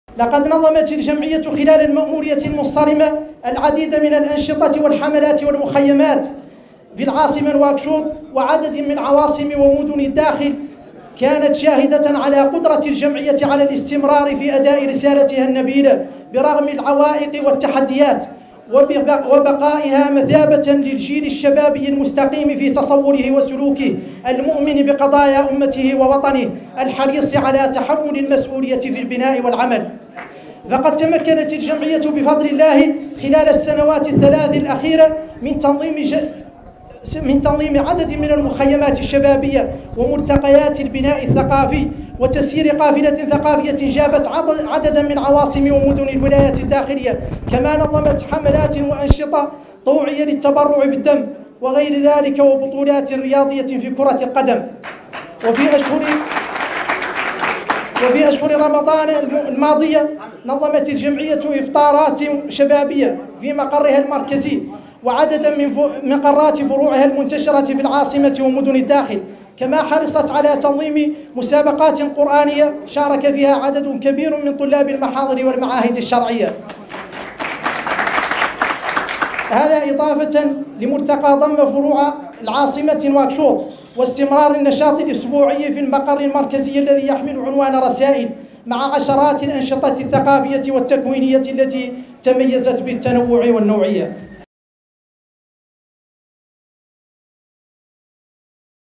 جمعية شبيبة بناء الوطن: تنظم مؤتمرها العادي الخامــ5ــس..
رئيس الجمعية الحسن ولد بدو في كلمته بالمناسبة قال: إن المأمورية الماضية شهدت تنظيم عشرات الأنشطة التطوعية والمخيمات الثقافية.
28/12/2019 م